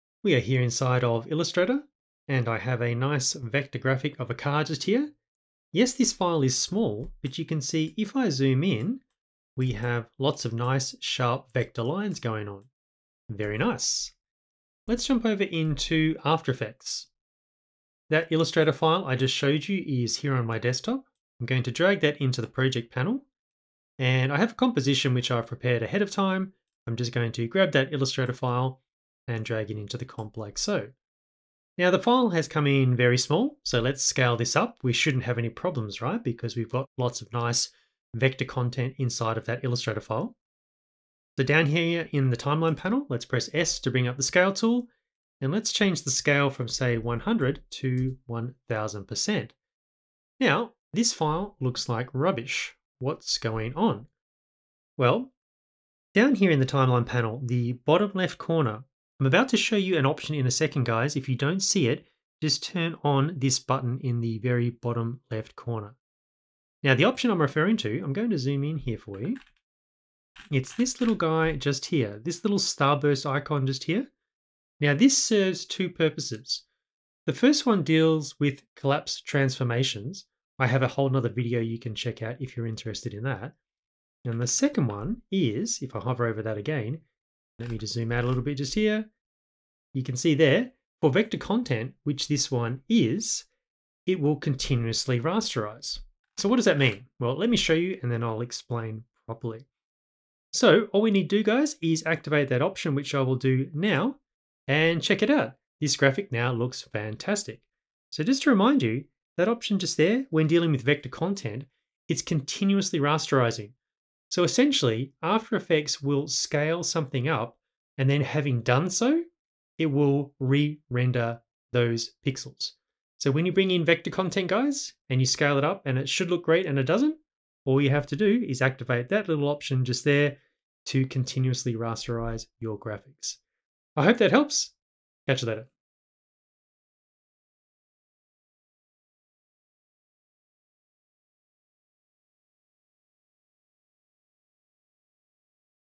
Stop Illustrator Vector Files Pixelating in After Effects (Continuously Rasterize)
Learn why Illustrator vector files pixelate when scaled up within After Effects and how to easily fix this issue. Details below...

Illustrator files containing only vector content effectively have no resolution. Why? Because such files can be scaled to any size while retaining crisp, clean vector edges. However, if such files are imported into After Effects and scaled up in size, they begin to look pixelated. Thankfully there is a switch that can be activated for such layers within the Timeline panel to remedy this situation. The icon for this switch resembles a star burst and for vector based layers it will force After Effects to re-render the layer after first honouring any upscaling of that layer.